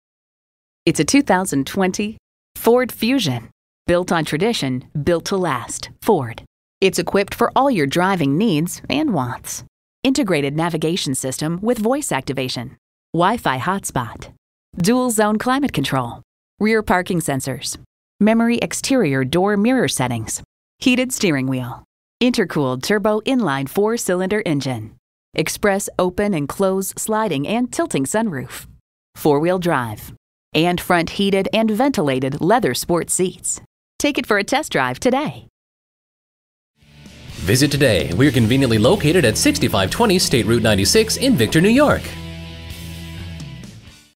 2020 Ford Fusion Rochester NY Victor, NY
Year: 2020
Make: Ford
Model: Fusion
Trim: Titanium
Engine: EcoBoost 2.0L I4 GTDi DOHC Turbocharged VCT
Transmission: 6-Speed Automatic
Color: Black
Mileage: 58005
Stock #: VT45313
VIN: 3FA6P0D97LR131127

Address:
6520 State Route 96